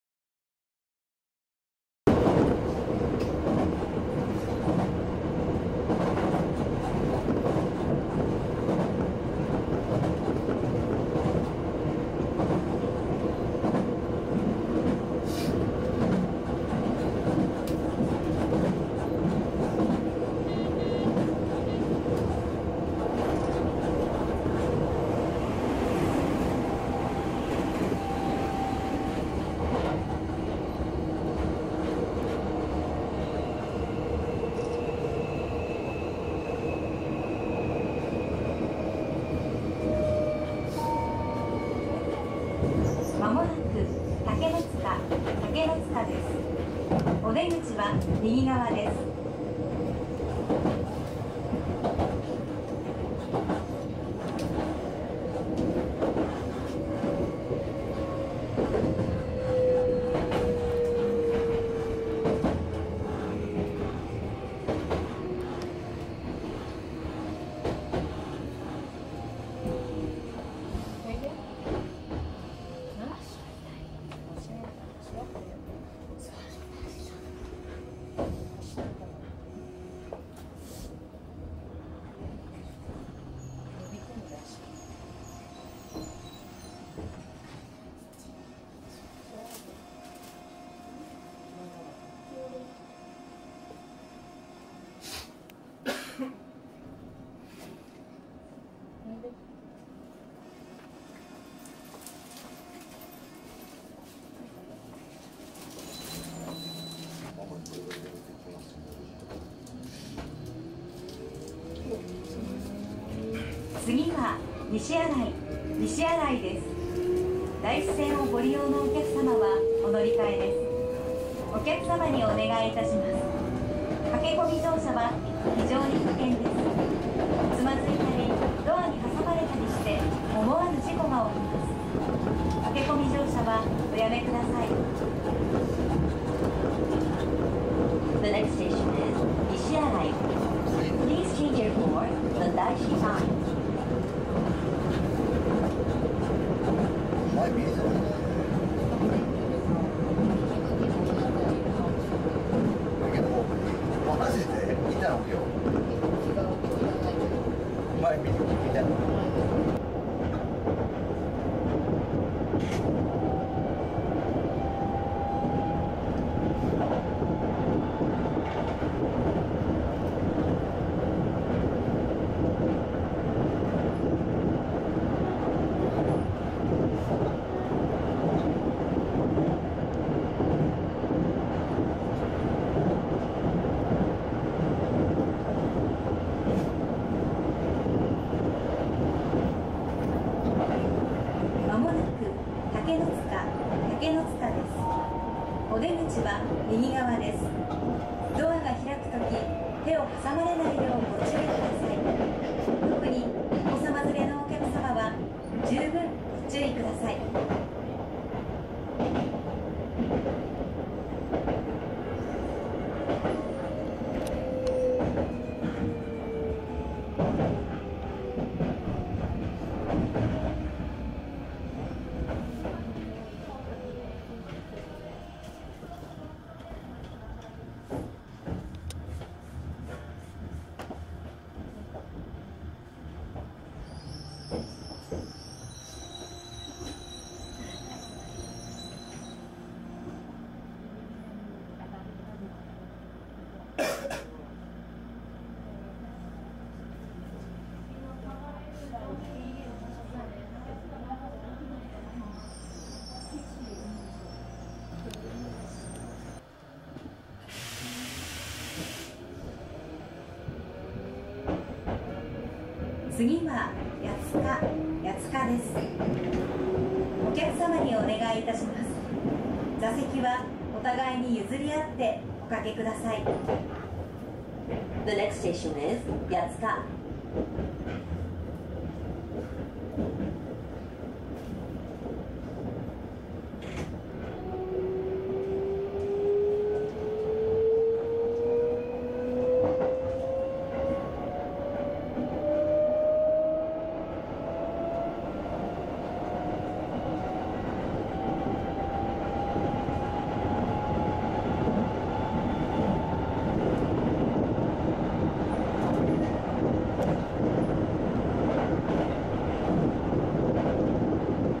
《2018.10》東武スカイツリーライン竹ノ塚駅付近高架化工事区間前面展望
東武スカイツリーライン竹ノ塚駅付近の高架化工事区間状況を上下緩行線アングルの前面展望で確認しました!!